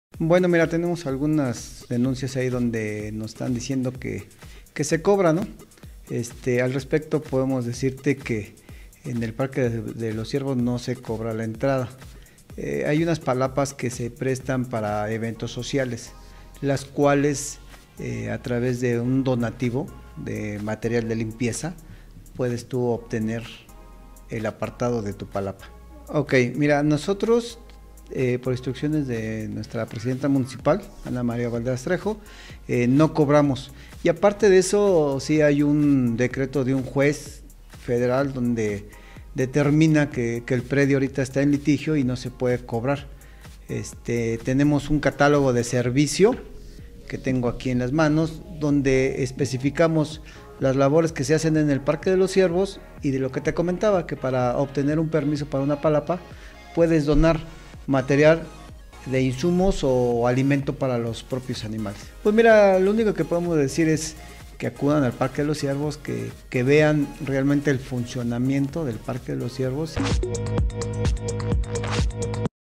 Mantiene Atizapán acceso sin costo al Parque de los Ciervos
El Director del Medio Ambiente en Atizapán, Ernesto Alonso, señaló que desde el inicio de la administración en el Parque de los Ciervos no se cobra dinero a los visitantes por accesar o por utilizar las palapas.

Explicó que para poder apartar alguna de las tres palapas el único requisito es donar alimentos o artículos de limpieza, mismos que se utilizan en el parque para su mantenimiento.

Además, sostuvo que otra de las razones por la cual no se cobra el acceso a éste sitio, es derivado de la orden de un juez federal ya que el predio se encuentra en litigio.